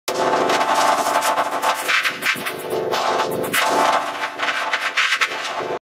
CHANGED Klasky Csupo in Videoup v400
I changed because MKD12 HD 2021 made a bad effects so I'll change it
How to make changed Videoup v400
Video: X takes a flu shot + Radioup v9
Audio: Radioup v2.NONE + IO-Phaser 16 Segements + Banjo Vocoder without vibrato + G Major 555 but replaced Ensemble to Bright